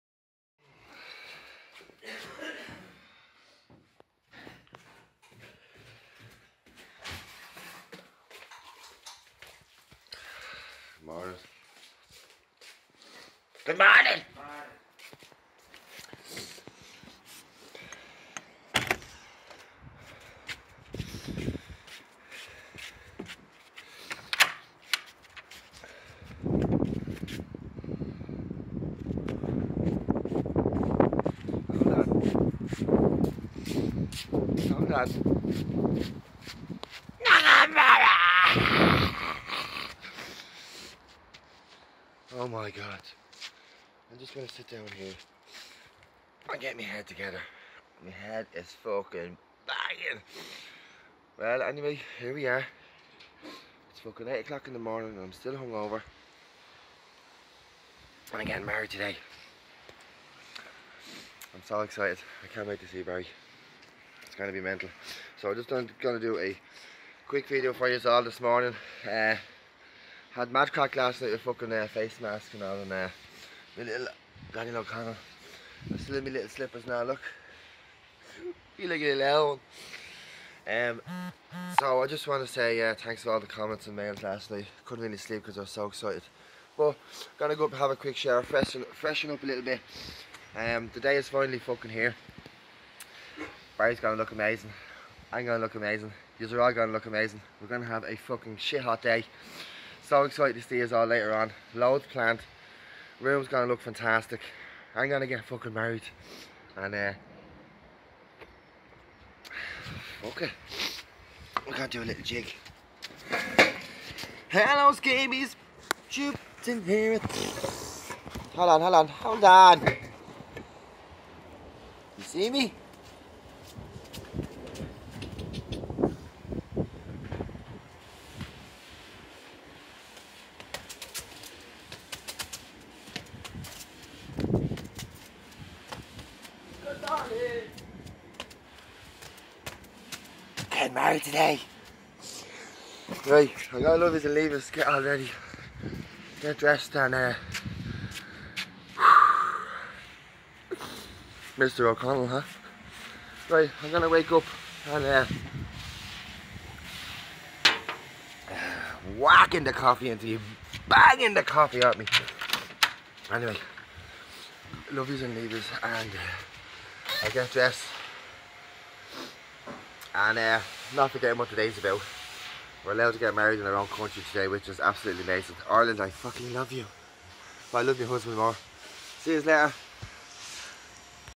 The morning of my Big Fat Irish Gay Wedding Day
So excited, hungover but so happy. Today I marry my best friend, lover and soul mate.. Thanks for all your wishes guys x